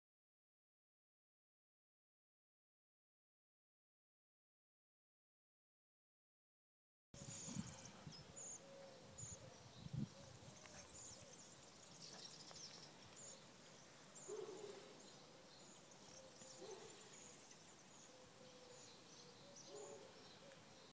The World's Strongest Ant
Ant carries twig more than twice its' size, Is this the world's strongest ant?